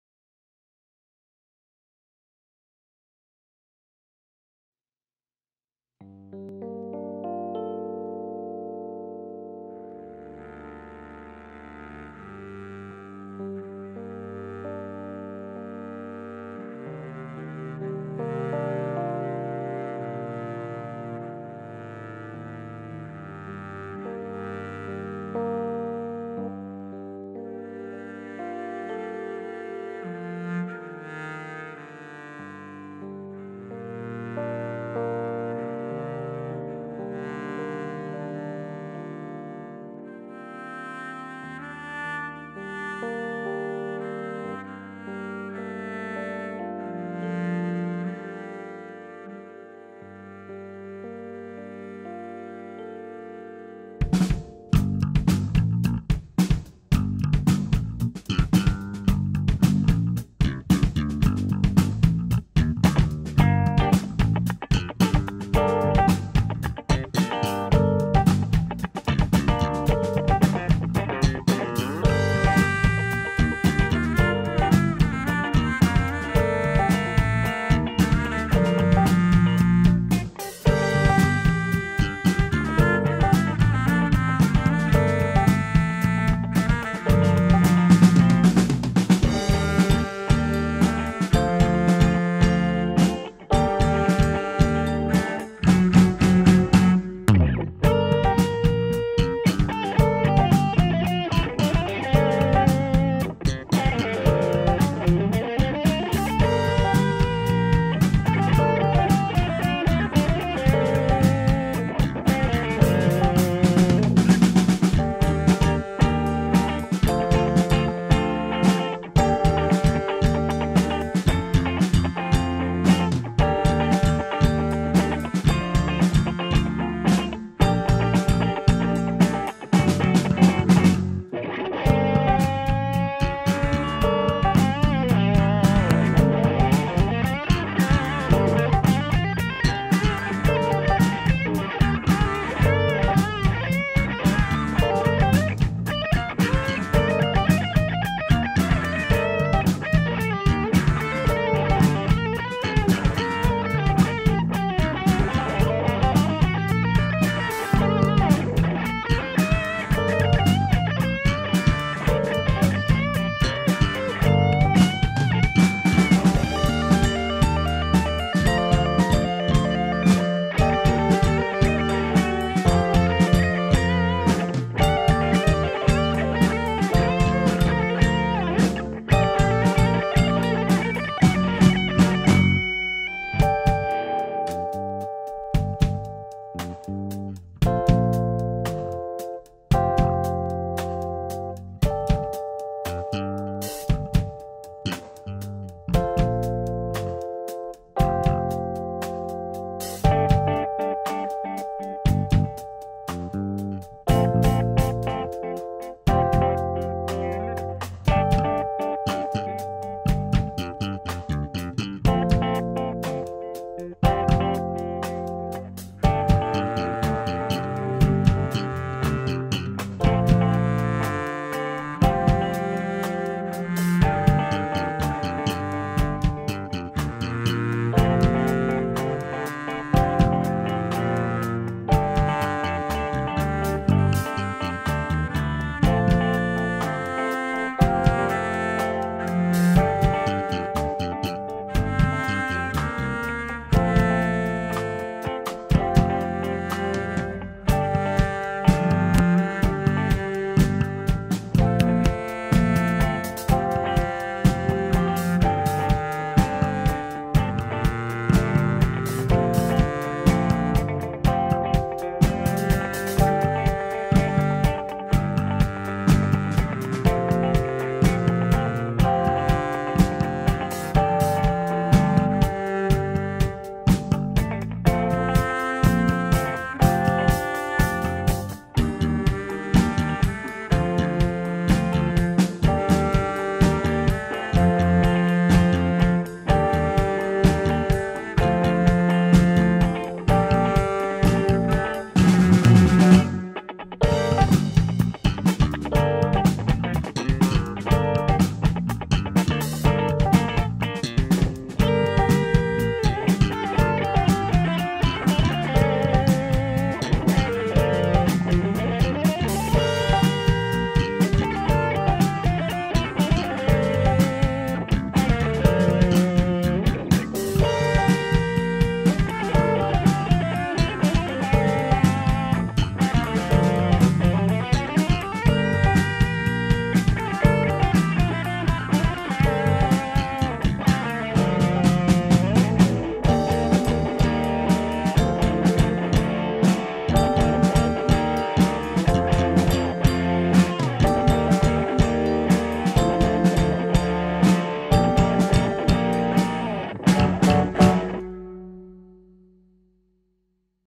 Musiques Actuelles - Atelier 1 du mercredi - Composition 1 - 26/05/2021
Un grand merci à La Sirène pour avoir accueilli l'enregistrement des élèves, et à la classe de Musique Assistée par Ordinateur (MAO) pour le mixage.
conservatoire.agglo-larochelle.fr